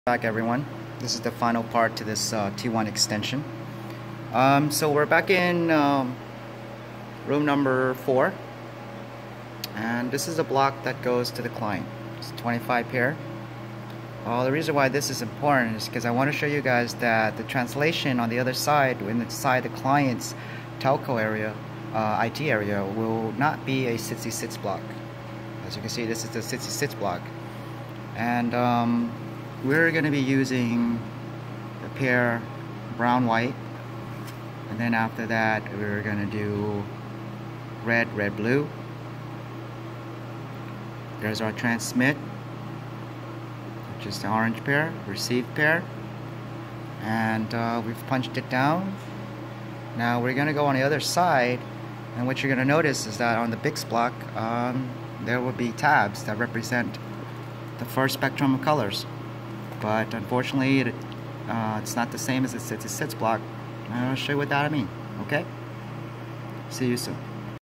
How to: T1 extension, Super Mall Multi Mode punch down.(6)
3 hops 2 methods of punch down and T1 pin out information.  Guide to Tx and Rx concept theory of ping and loopback, rj45 efficient use of line and jack.